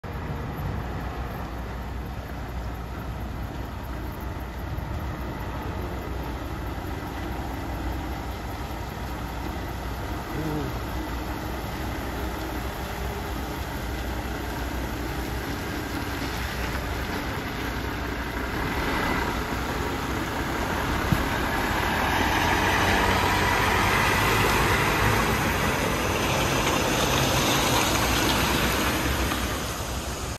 츄레라(트레일러) 지나간다
차고지에서 츄레라(트레일러) 출발합니다.